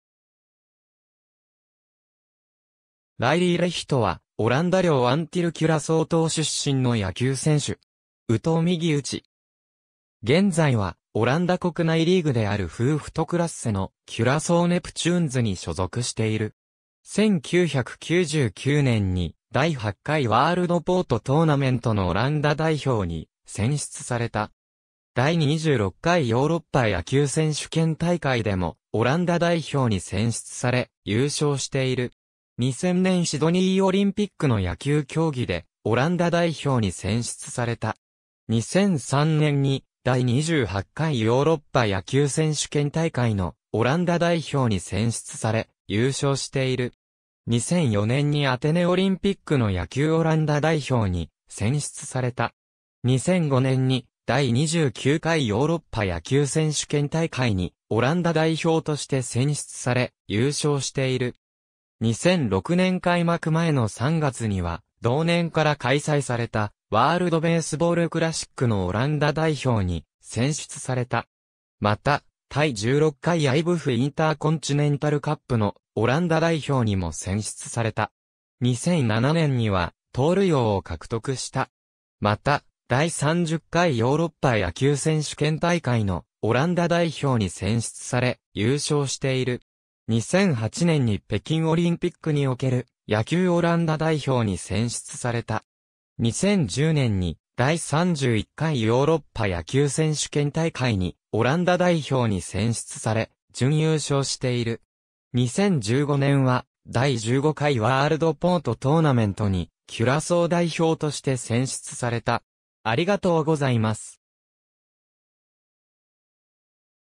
ライリー・レヒト
ワールド・ベースボール・クラシック・オランダ代表選手
ライリー・レヒト（Reily Legito, 1978年7月26日 - ）は、オランダ領アンティル・キュラソー島出身の野球選手(遊撃手・三塁手)。
右投右打。
現在は、オランダ国内リーグであるフーフトクラッセのキュラソー・ネプチューンズに所属している。
1999年に第8回ワールドポート・トーナメントのオランダ代表に選出された。
第26回ヨーロッパ野球選手権大会でもオランダ代表に選出され、優勝している。
2000年シドニーオリンピックの野球競技でオランダ代表に選出された。
2003年に第28回ヨーロッパ野球選手権大会のオランダ代表に選出され、優勝している。
2004年にアテネオリンピックの野球オランダ代表に選出された。
2005年に第29回ヨーロッパ野球選手権大会にオランダ代表として選出され、優勝している。
2006年開幕前の3月には、同年から開催されたワールドベースボールクラシック(WBC)のオランダ代表に選出された。
また、第16回IBAFインターコンチネンタルカップのオランダ代表にも選出された。
2007年には盗塁王を獲得した。
また、第30回ヨーロッパ野球選手権大会のオランダ代表に選出され、優勝している。
2008年に北京オリンピックにおける野球オランダ代表に選出された。
2010年に第31回ヨーロッパ野球選手権大会にオランダ代表に選出され、準優勝している。
2015年は第15回ワールドポート・トーナメントにキュラソー代表として選出された。